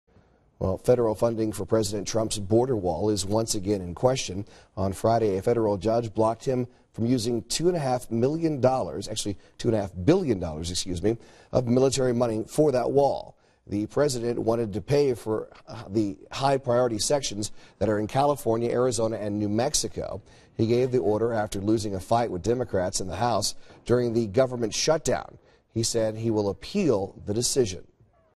Federal Judge Blocks Border Wall Funding
A judge blocks the budget for a border wall.